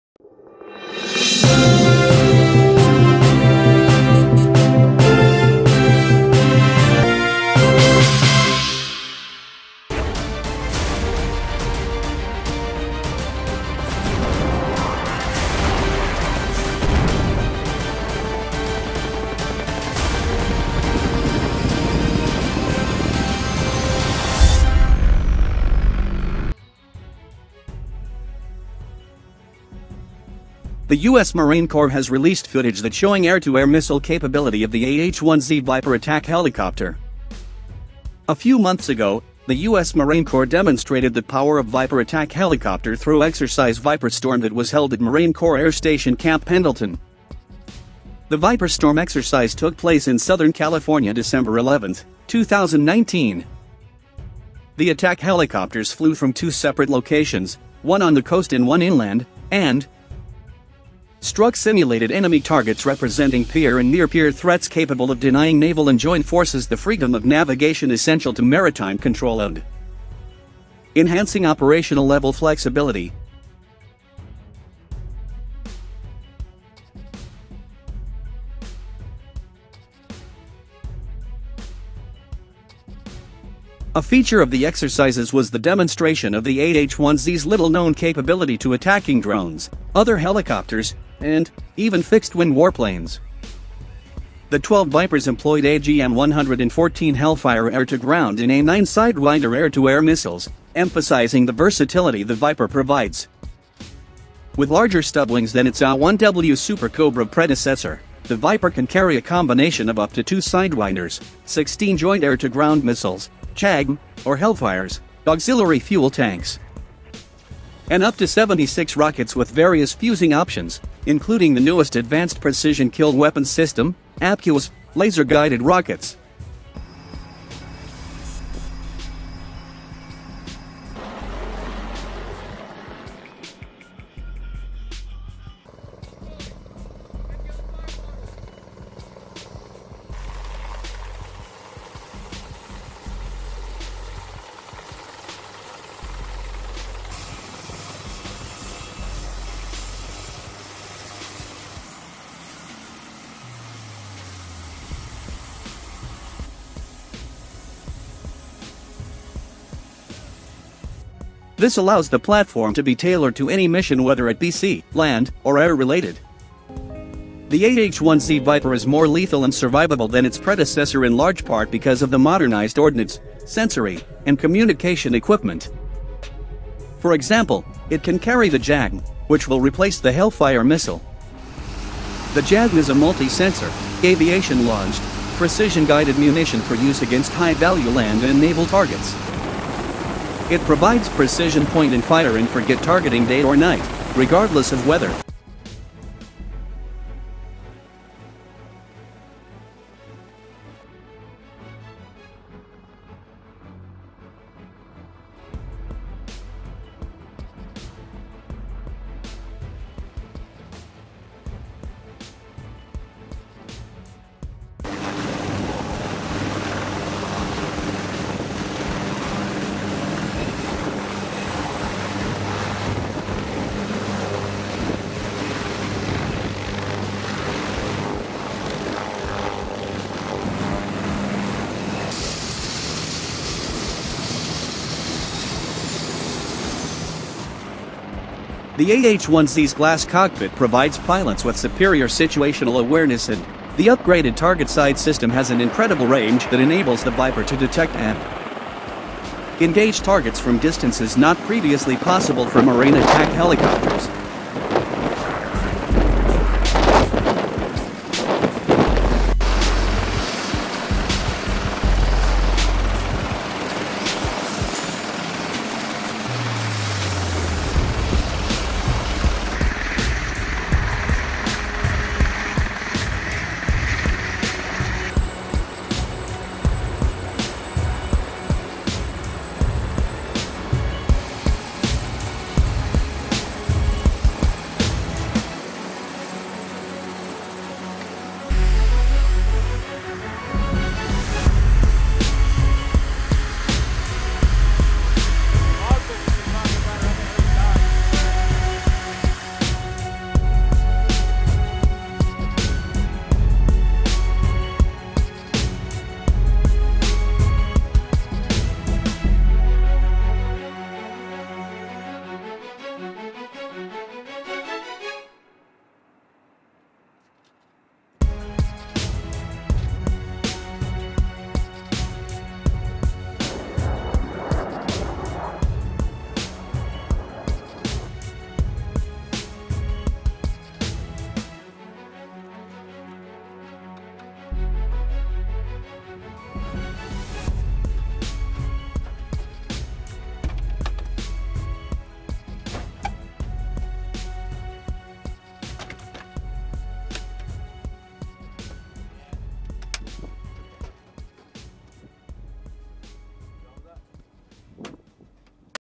Viper AH-1Z attack helicopter air missile capability
The AH-1Z Viper is more lethal and survivable than its predecessor in large part because of the modernized ordnance, sensory, and communication equipment. For example, it can carry the JAGM, which will replace the Hellfire missile. The JAGM is a multi-sensor, aviation-launched, precision-guided munition for use against high-value land and naval targets. It provides precision point and fire-and-forget targeting day or night, regardless of weather.